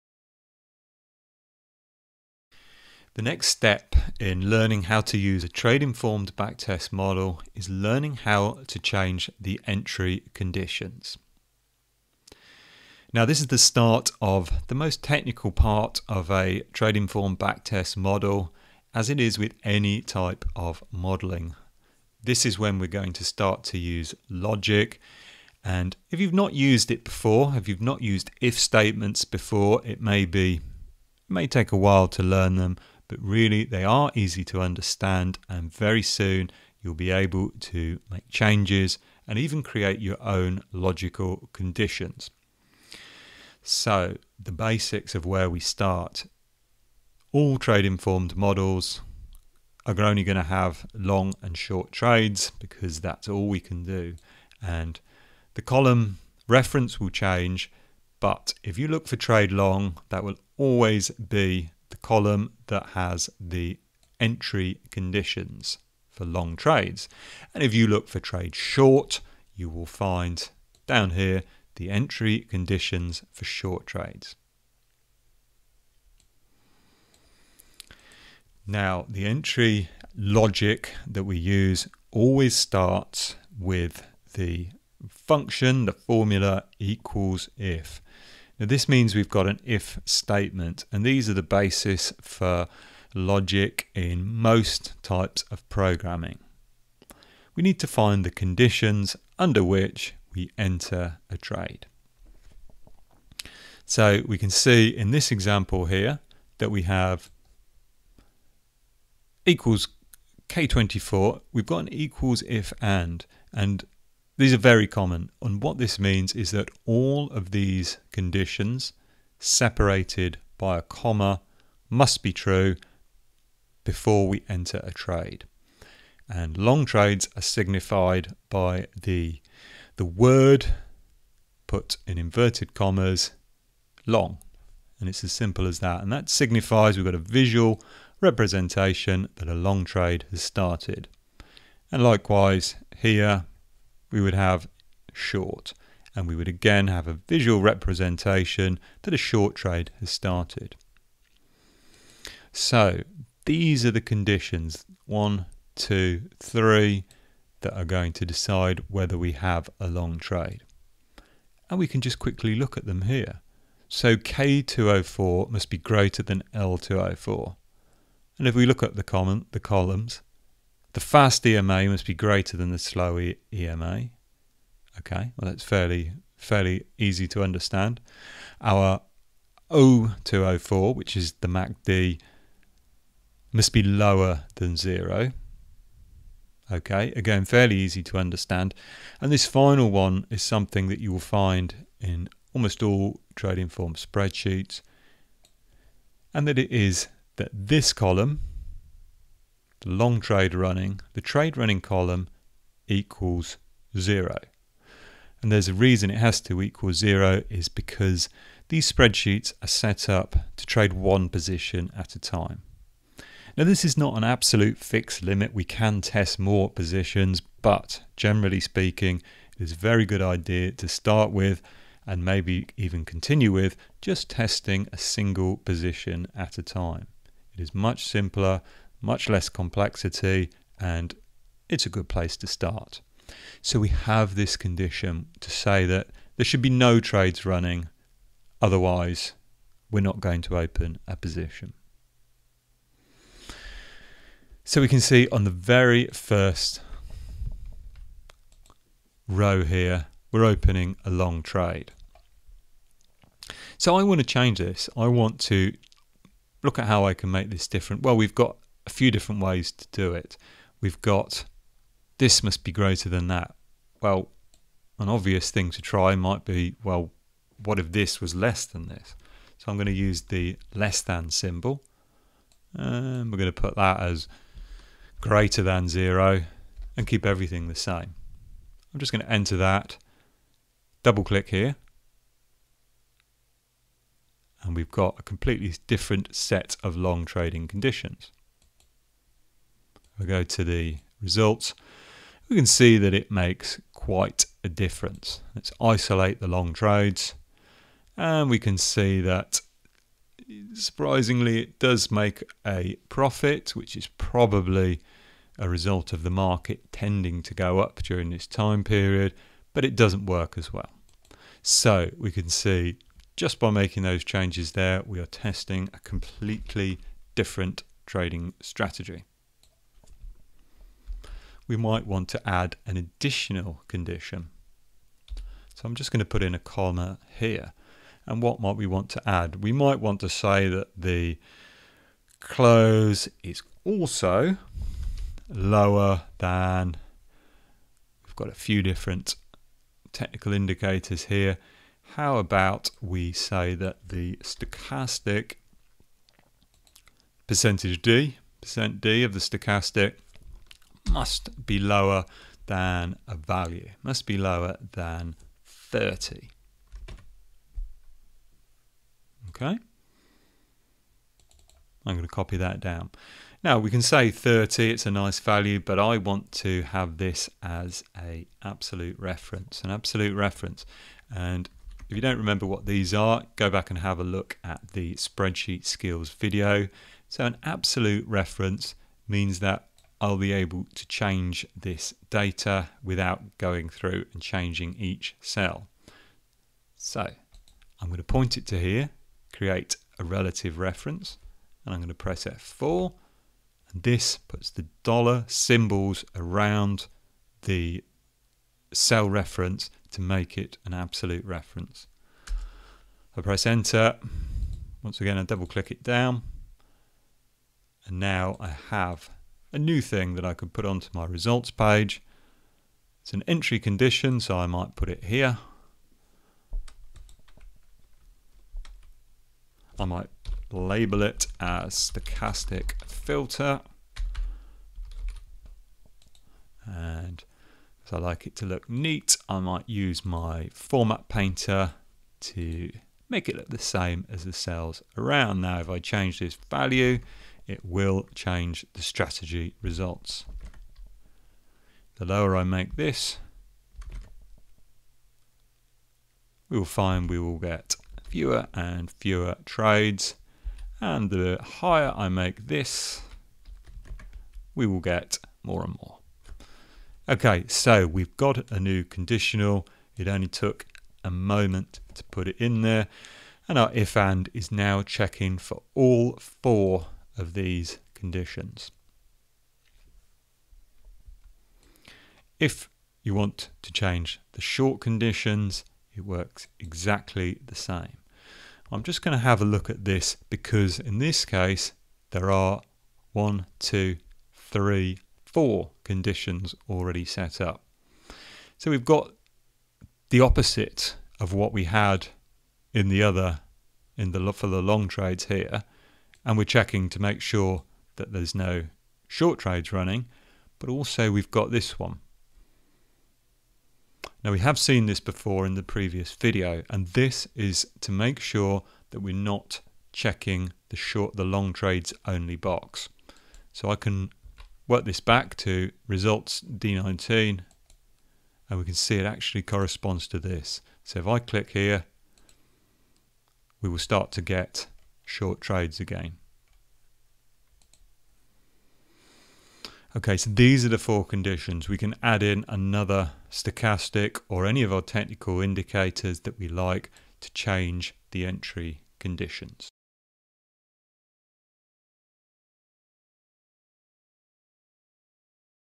How to Change the Entry Conditions in a Tradinformed Backtest Model
Video guide explaining how you can change the entry conditions in a Tradinformed model. In this video, I explain IF Statements, AND and OR.